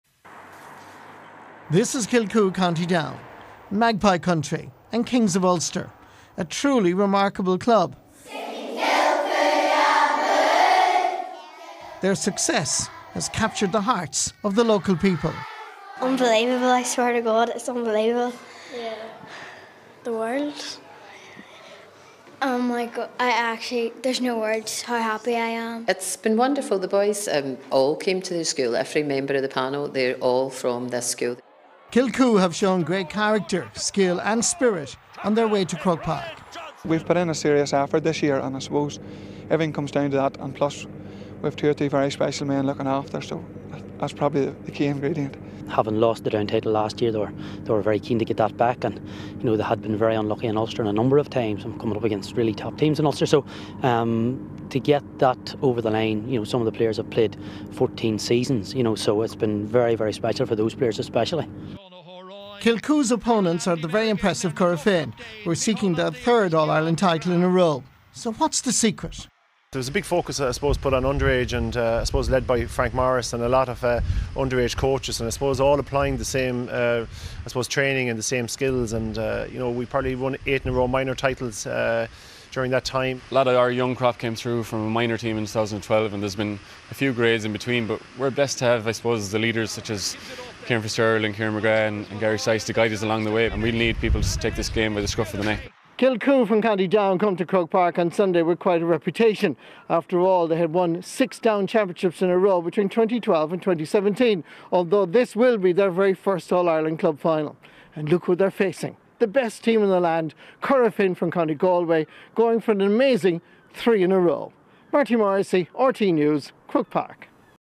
Kilcoo v Corofin
Ahead of the weekend's All-Ireland club football final, Marty Morrissey visits Kilcoo, who are hoping to win their first ever title